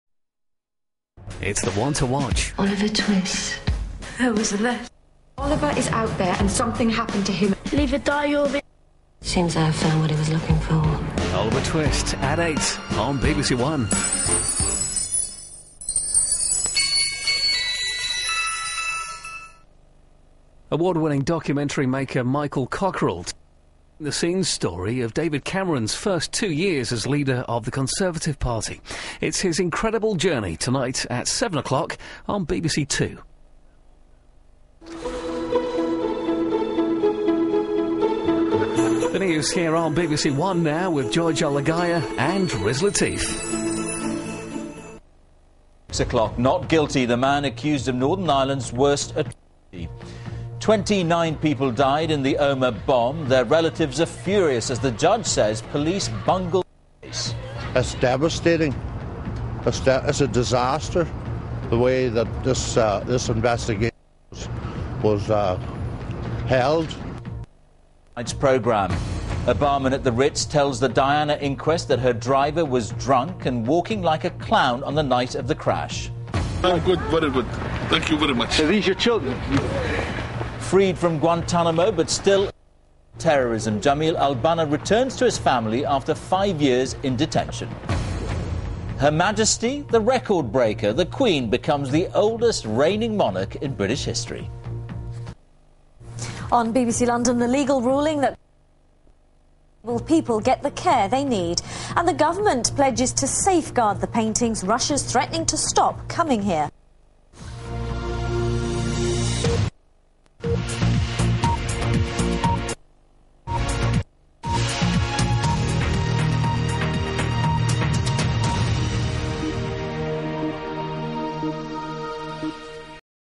BBC1 Junction 2007 (4)
A trail, a slide, and an ident going into the news.